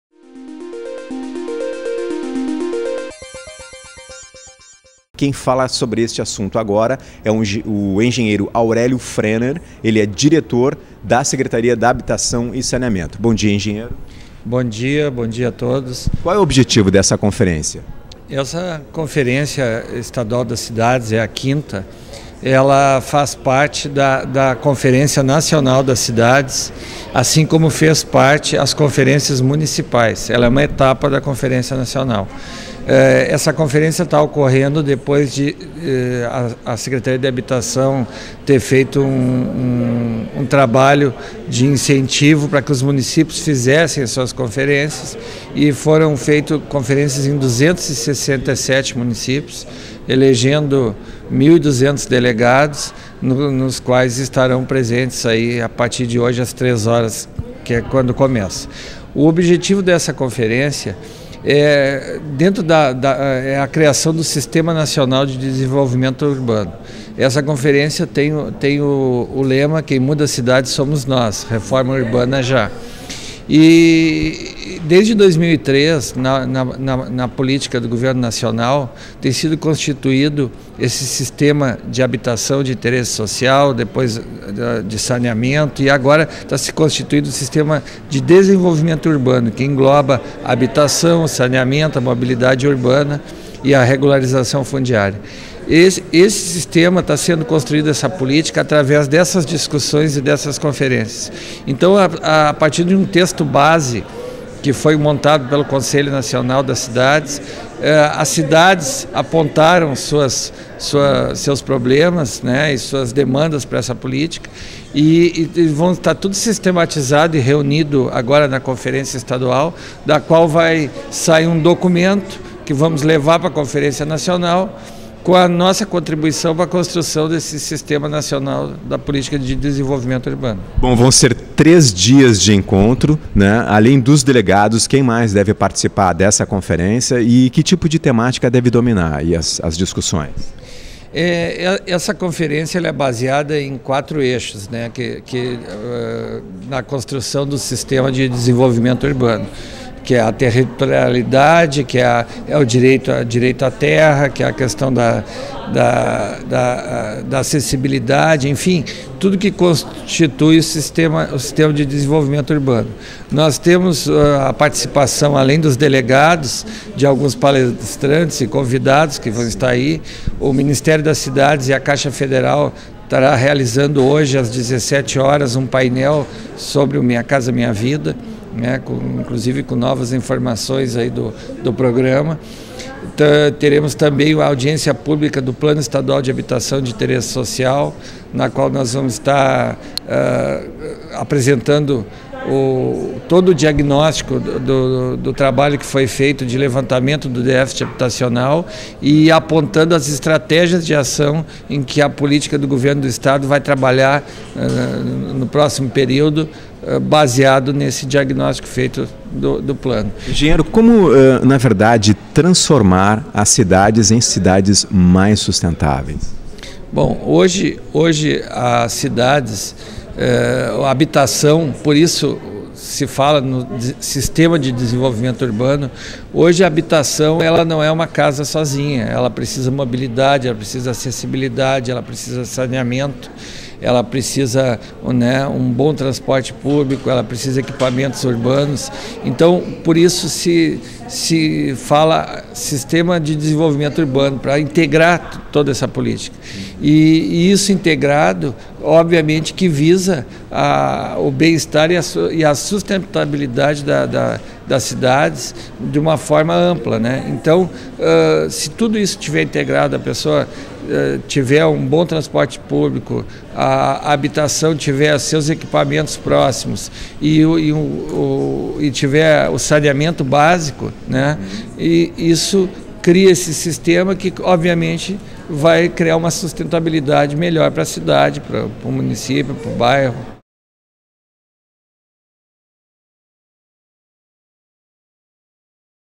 5º Conferência Estadual das Cidades debate assuntos relacionados a reforma urbana
Acontece na tarde desta quinta-feira (15), no Centro de Eventos da Fiergs, a 5º Conferência Estadual das Cidades, que nesta edição tem como lema " Quem muda a cidade somos nós: reforma urbana já".